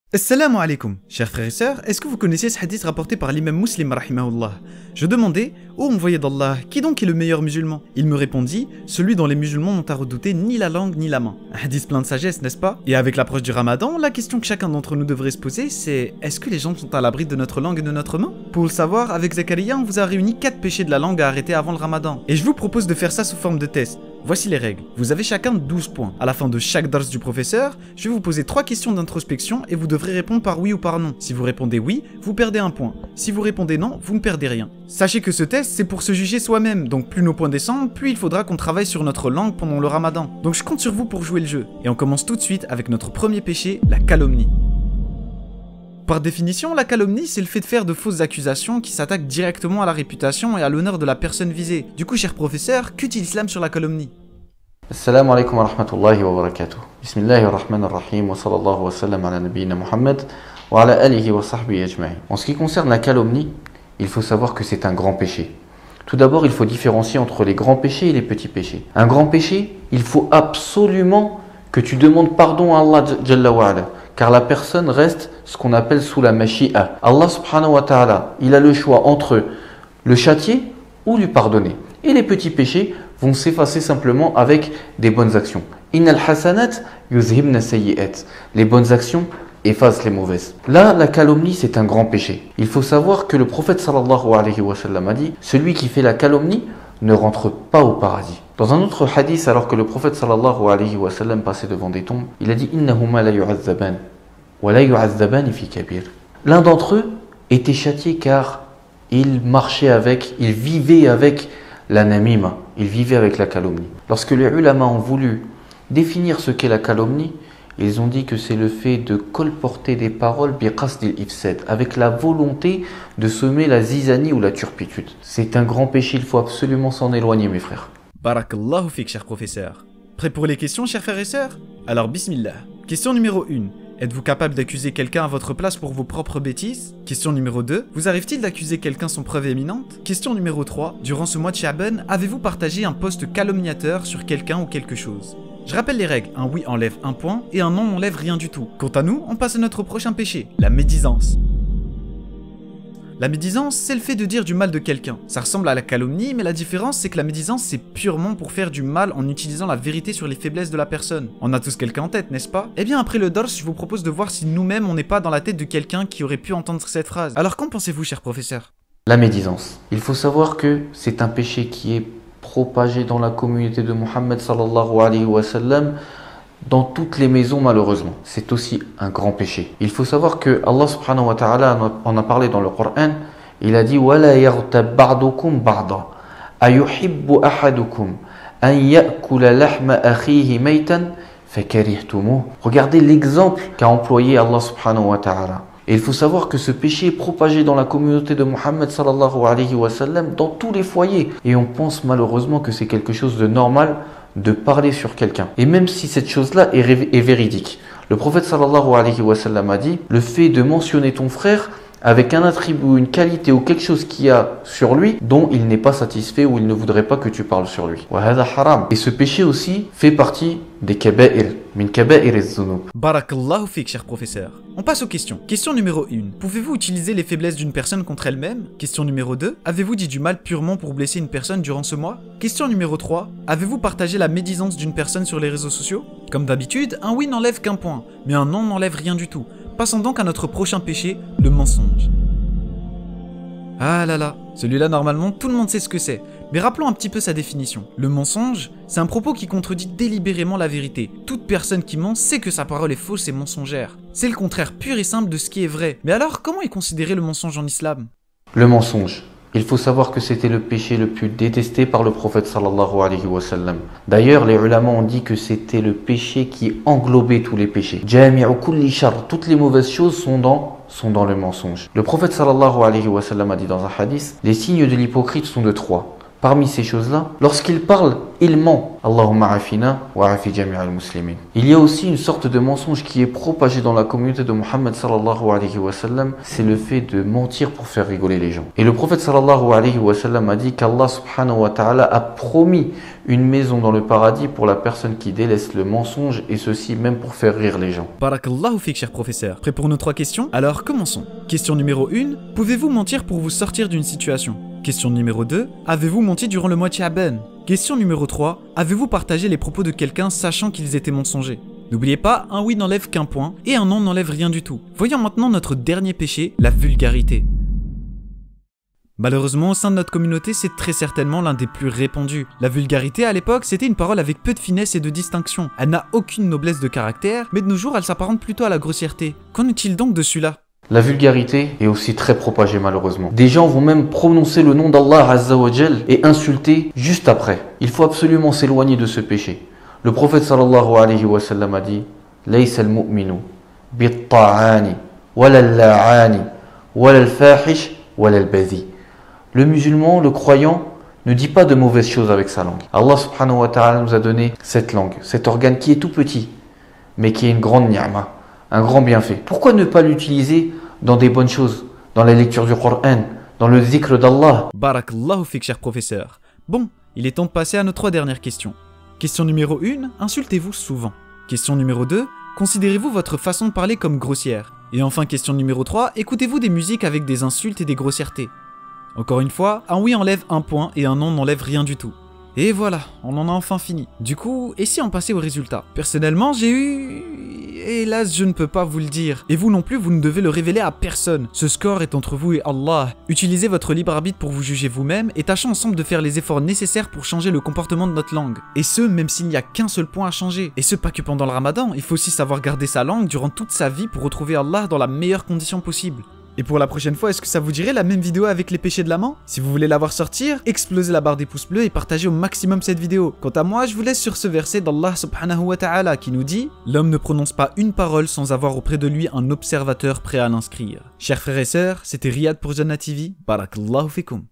⚠ CES 4 PÉCHÉS QUE TOUT LE MONDE IGNORE ⚠
Qu'est ce que la médisance et la calomnie ? Es-tu vulgaire et profères tu des mensonges ? Comment utilises tu la langue qu'Allah t'a donné ?

Chez Jannah TV on te teste !

A travers cette vidéo, nous allons ensemble effectuer un quizz d'introspection afin de savoir si nous vouons notre vie au bien de l'islam et de la communauté du prophète (saws) à travers le comportement de notre langue. Les péchés de la langue que nous allons voir sont : La calomnie, la médisance, le mensonge et la vulgarité qui sont les plus redondants au sein de la communauté musulmane. En tant que croyant de la oummah, il est de notre devoir de bien représenter notre religion à travers l'éloquence de nos propos.

0:20 : Règle du jeu
0:43 : Le péché de la calomnie
2:31 : Le péché de la médisance
4:00 : Le péché du mensonge
5:15 : Le péché de la vulgarité

4 péchés de la langue à arrêter pendant le Ramadan.